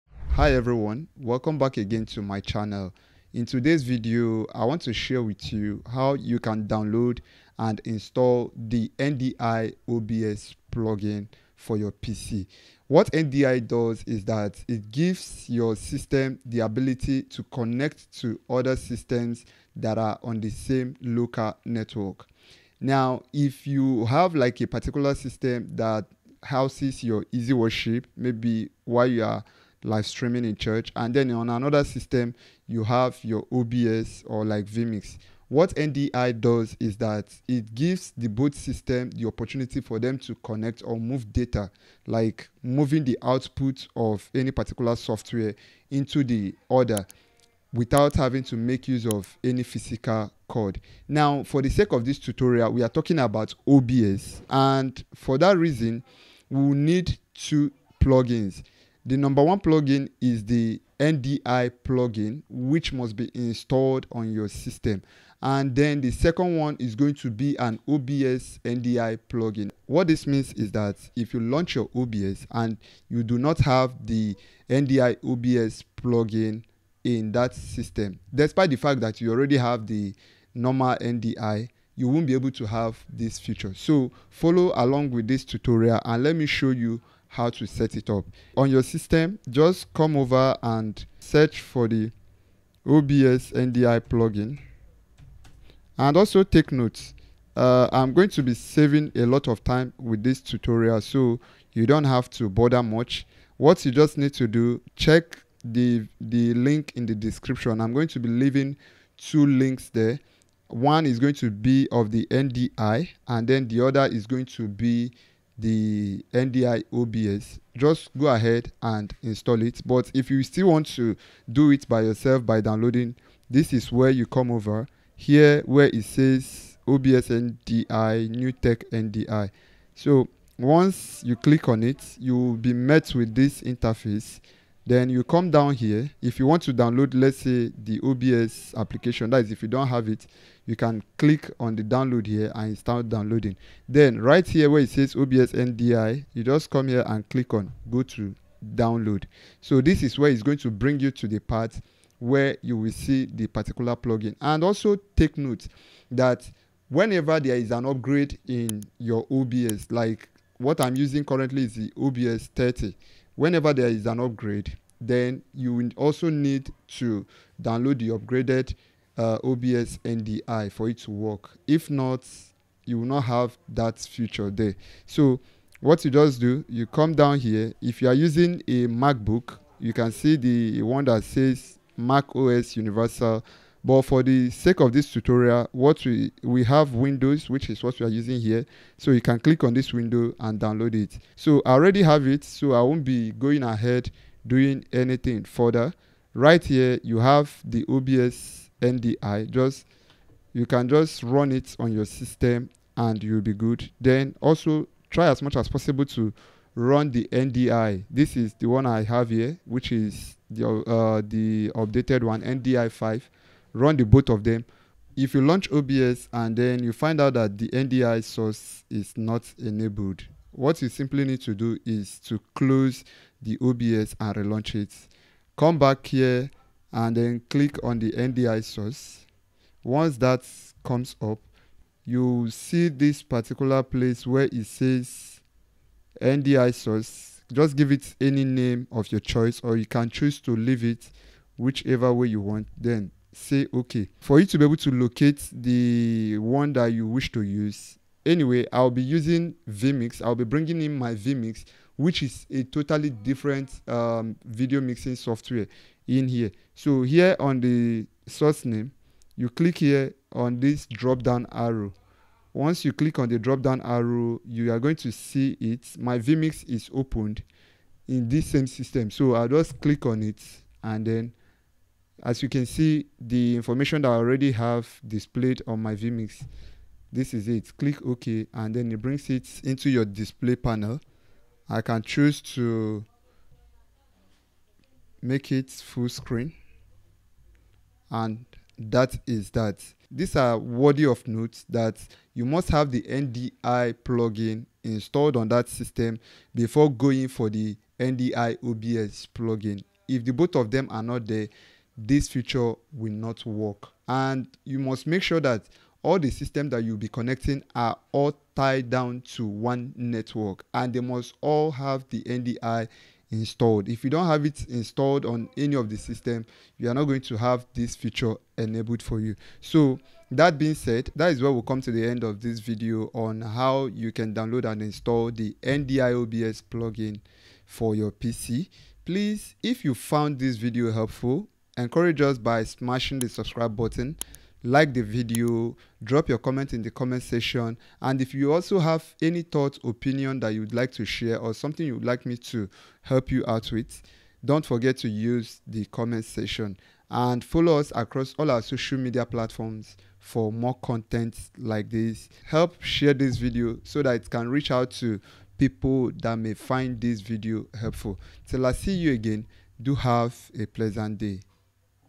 How to Download & Install OBS NDI Plugin on PC (2024) | Easy Step-by-Step Guide for Streamers
In this video, I'll show you how to easily download and install the OBS NDI plugin on your PC. The OBS NDI plugin is a powerful tool that allows you to connect multiple devices to your OBS Studio for seamless streaming and recording. Whether you're a beginner or an experienced streamer, this step-by-step guide will help you set up the OBS NDI plugin without any hassle.